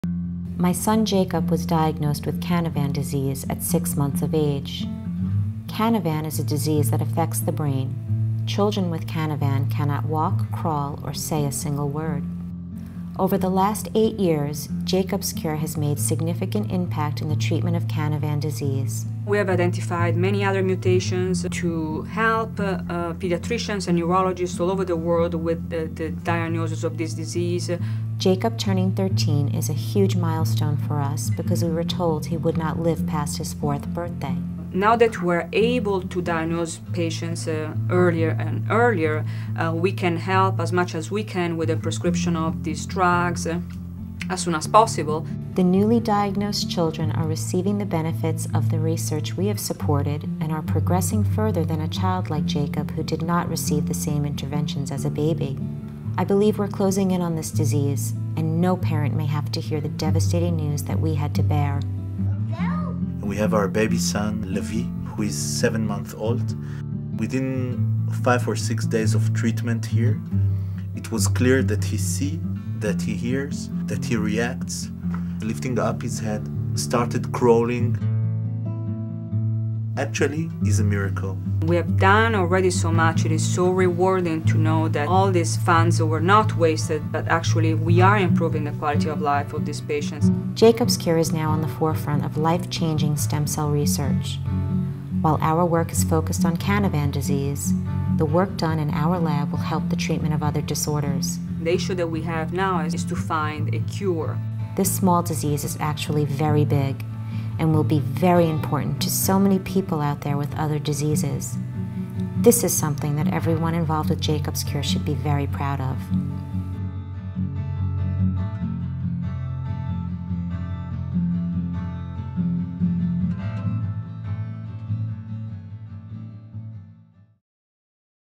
The Jacob's Cure Story
An informative 2-minute video about Canavan disease and the work Jacob's Cure has done in treating this disease over the past ten years.